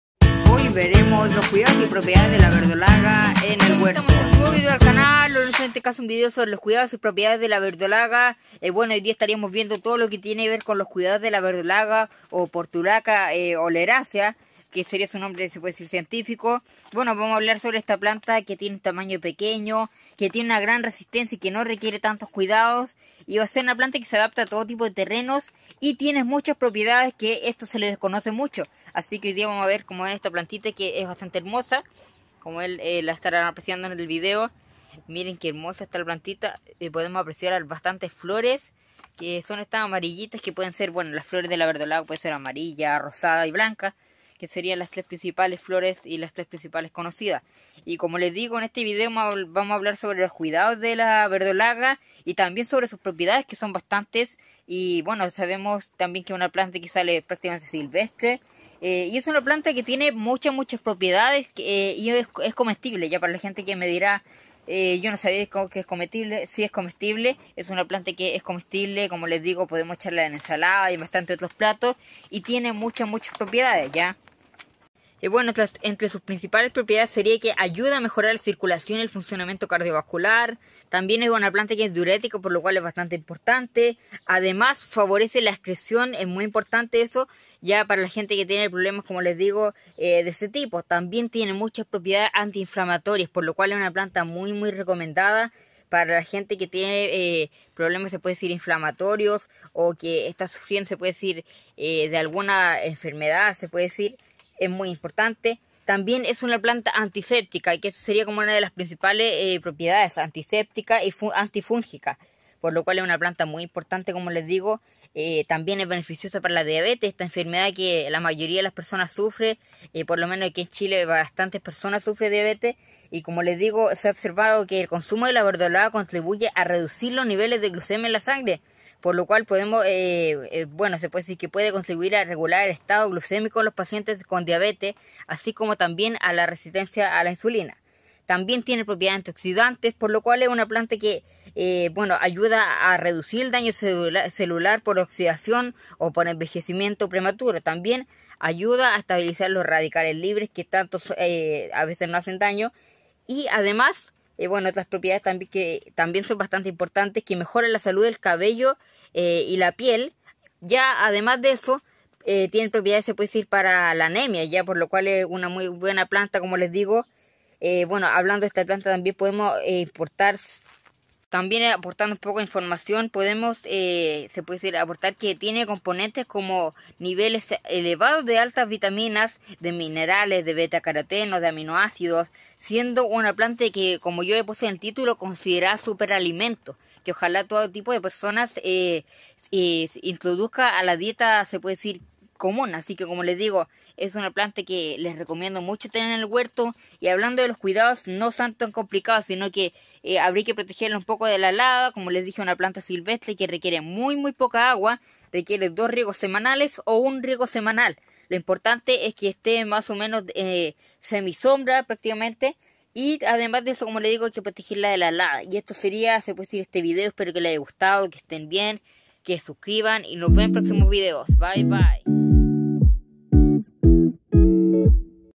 Los cuidados y propiedades de la verdolaga|(Portulaca oleracea)|super alimento|6TaTemporada
En este vídeo aprenderás las propiedades y cuidados de la planta de la verdolaga.
Propiedades como:
Planta para ayudar a tratar la resistencia a la insulina,Ayudar a mantener niveles glucémicos normales,Poseer propiedades antianemicas,Estás y otras propiedades bienen acompañadas de sus componentes como:Poseer un gran contenido de aminoácidos,Poseer una gran cantidad de vitaminas,entre otras.
Espero le guste este vídeo y lo compartan ya que sería de gran ayuda para informar a la gente de las propiedades de esta planta que es considera maleza
saludos de la huerta de Rafael